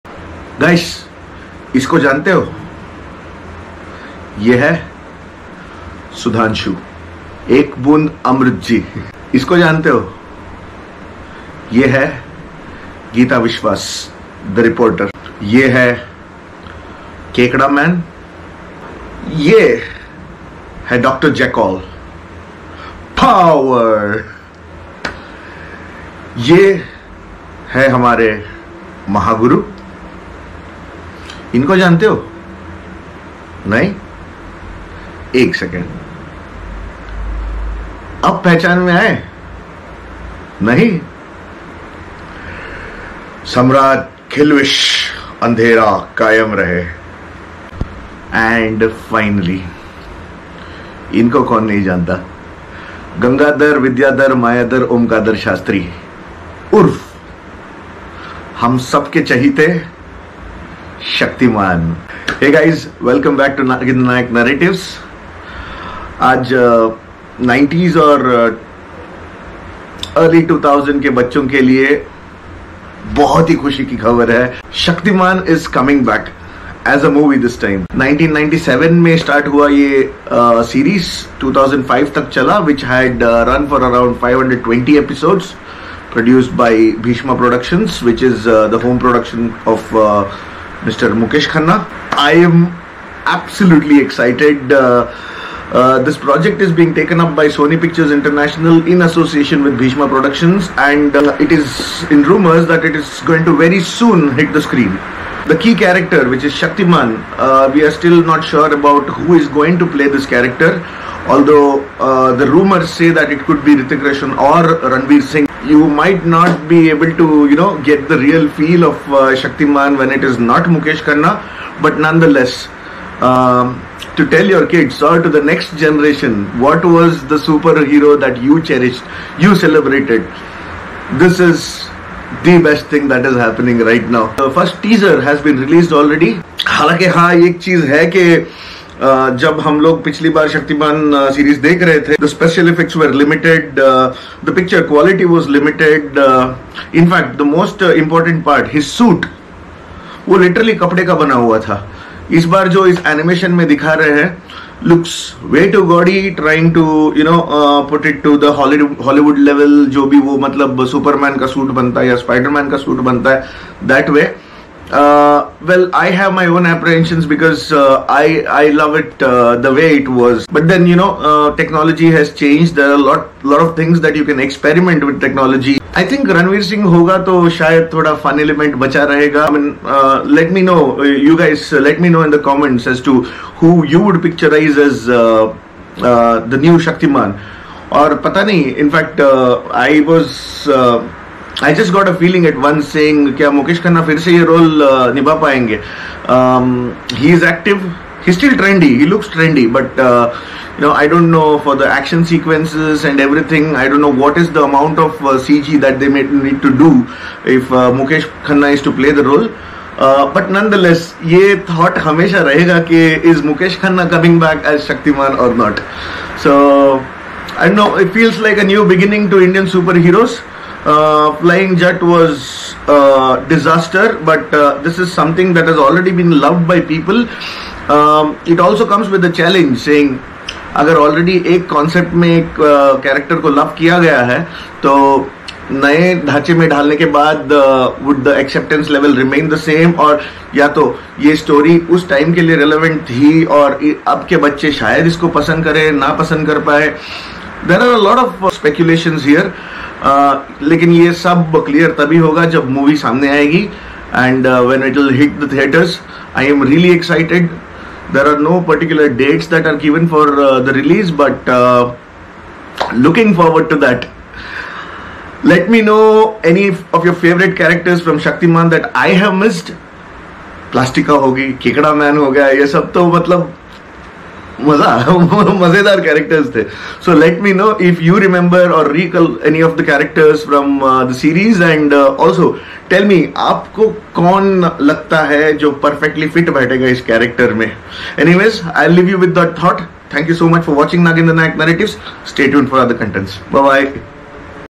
Kaun banega next SHAKTIMAAN| Shaktimaan movie teaser| About Shaktimaan
As Shaktimaan movie teaser is on air, the world has only one question. Who is the next shaktimaan? Could it be Hrithik, could it be Ranveer, could it be tiger Shroff or could it be Mukesh Khanna himself? We will have to wait a while more for the answer.